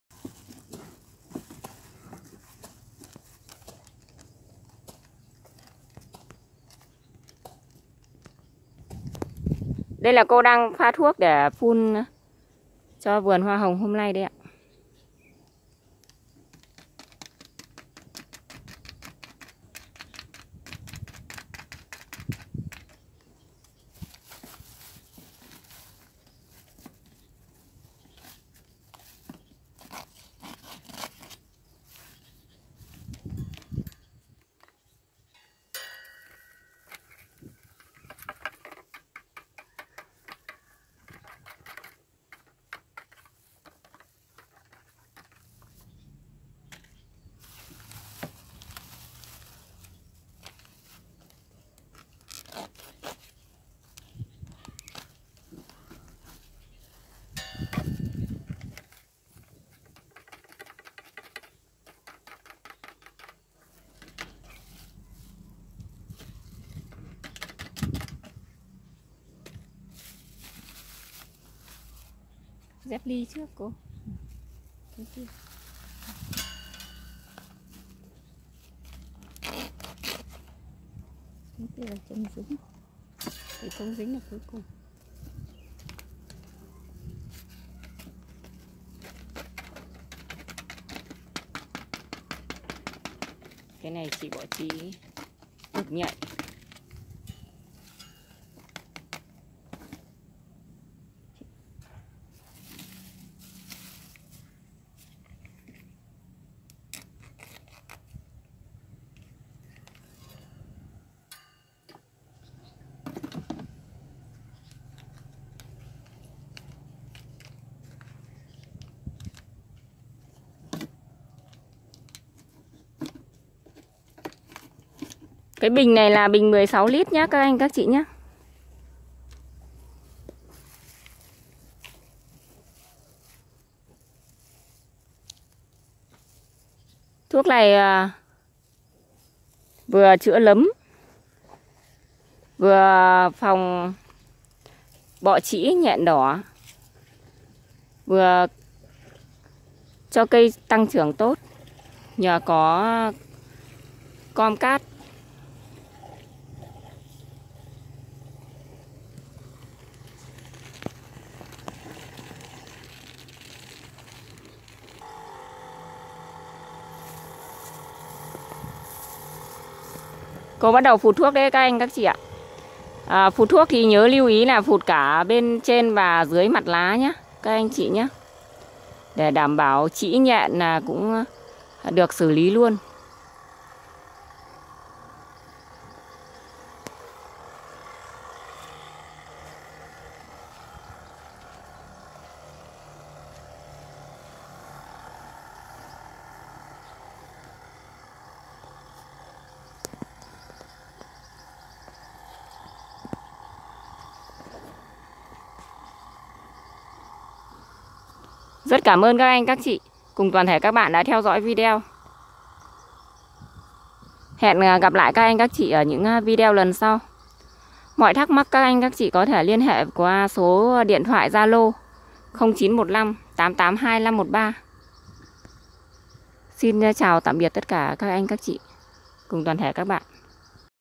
Phun thuốc phòng trừ bệnh nấm, bọ trĩ và nhện đỏ cho hoa hồng
Hoa hồng thường gặp phải nấm bệnh gây vàng lá khi mùa mưa. Nhà vườn hôm nay tiến hành xịt thuốc nấm và đồng thời xịt luôn bọ trĩ và nhện đỏ trên cây hoa hồng. Mời các anh chị và các bạn cùng tham khảo nhé. Đt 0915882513